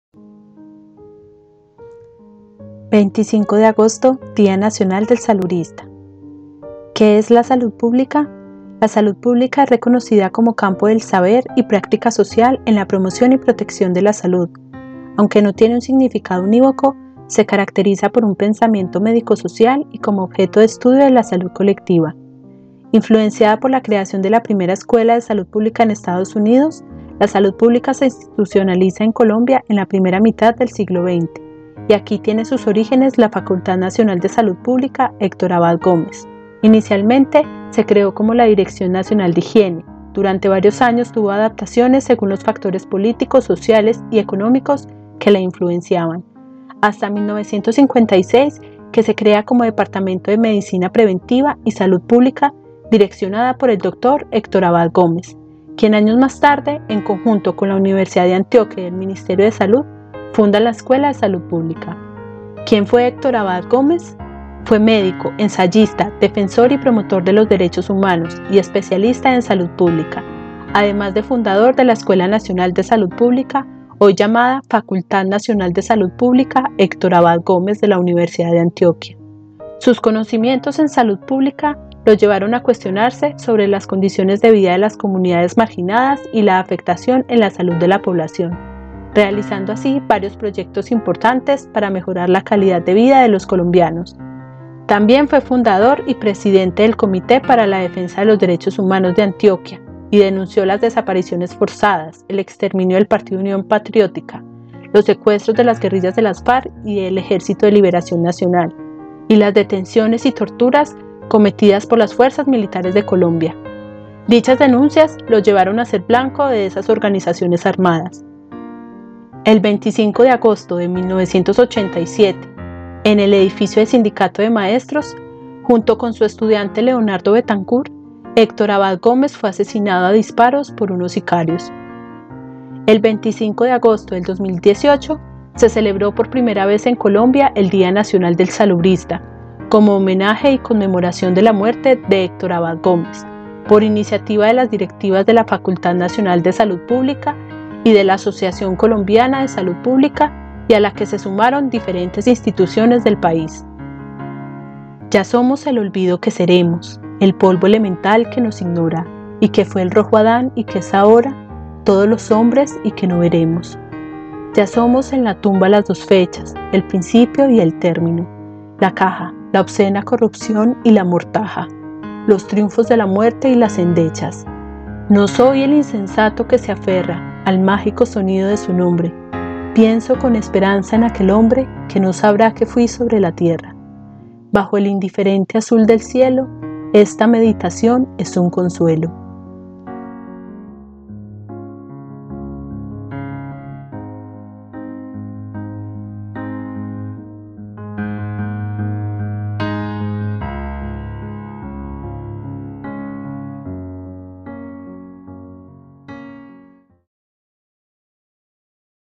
Comenzamos a celebrar el Día Nacional del Salubrista 2022
🤩¡Este mes celebramos el Día Nacional del Salubrista!

Queremos compartir contigo este video realizado por Mary Luz Pérez Patiño, una estudiante de nuestra Facultad quien tomó como base el documento creado por el profe Gabriel Otálvaro llamado El Día Nacional del Salubrista. Notas acerca del reconocimiento de una praxis social necesaria.

👉Conoce el documento aquí: t.ly/xLoA